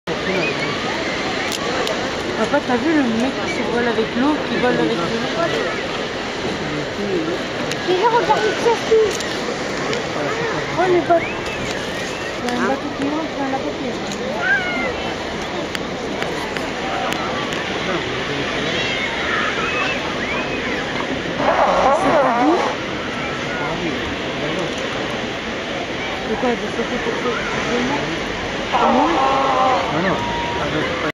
Nazare # Portugal # FLYBOARD im MEER !!
Wasserspaß in Nazare mit dem FLYBOARD ! 9/2015